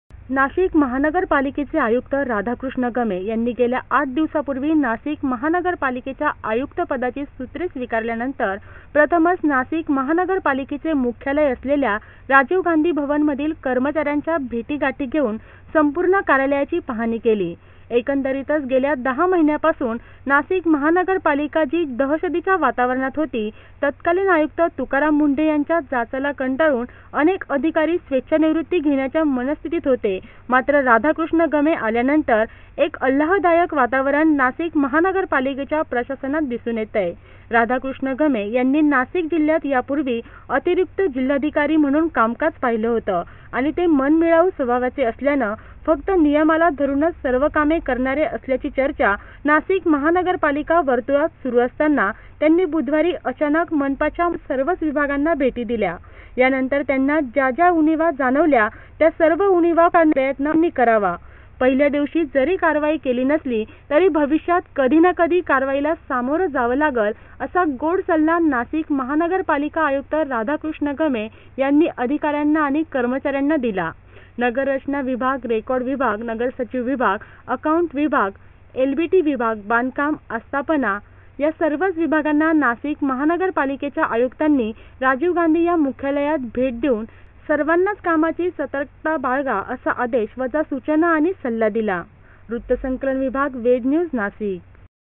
वेध न्यूज -
पदभार स्वीकारल्यानंतर बुधवारी आयुक्तांनी केली पाहणी; विविध विभागाच्या अधिकाऱ्यांना उणीवा भरून काढण्याची सूचना